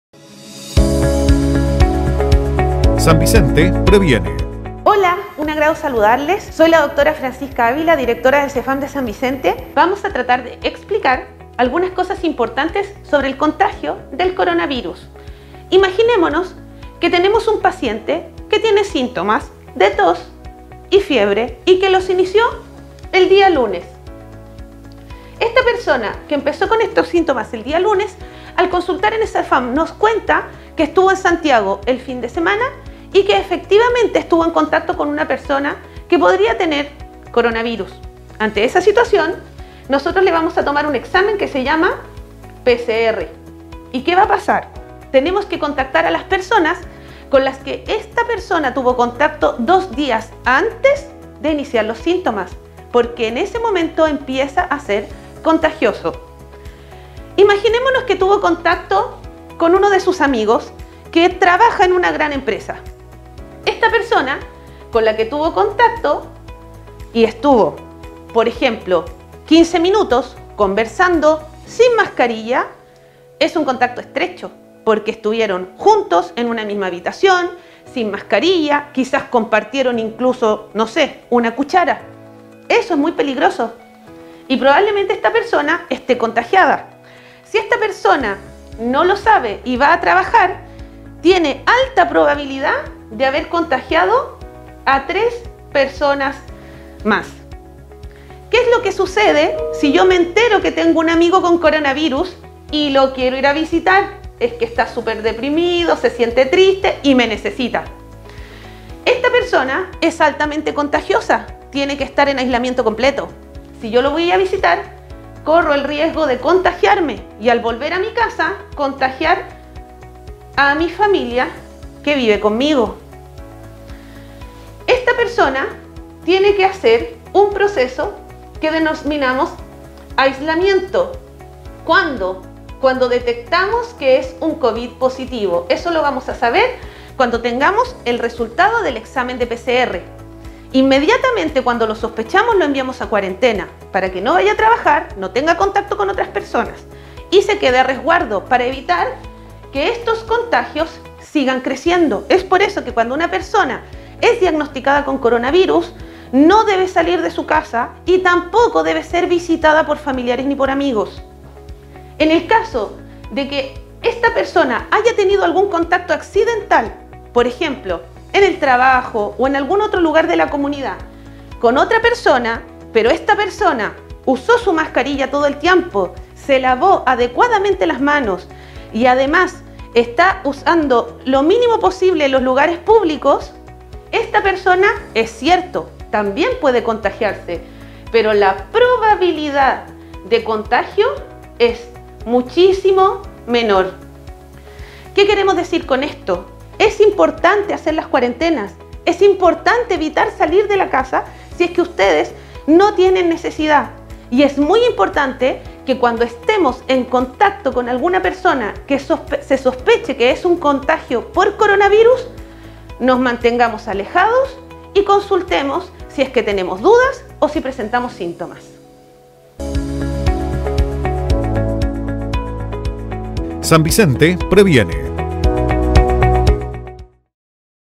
DIRECTORA DE CESFAM EXPLICA CÓMO ROMPER LA TRASMISIÓN DEL CONTAGIO
La directora del Cesfam de San Vicente, Dra. Ávila Chamorro, explica en el siguiente vídeo el ciclo de contagio de COVID-19 y cómo desarrollan los equipos de salud el seguimiento y la trazabilidad de os casos, lo que permiten limitar la propagación del virus.

También se refiere a conceptos como contacto estrecho, cuarentena y aislamiento, este último, una de las principales herramientas de prevención, por lo tanto, es fundamental que las personas confirmadas con COVID-19 positivo, no salgan de su casa y tampoco sean visitadas, así como también es importante que consulten a un médico si tienen alguna duda o presentan síntomas.

Como siempre, la Dra. Ávila llama a seguir practicando todas las medidas dadas a conocer para la prevención, distanciamiento físico, lavado frecuente de manos, uso correcto de la mascarilla y en lo posible, quedarse en casa.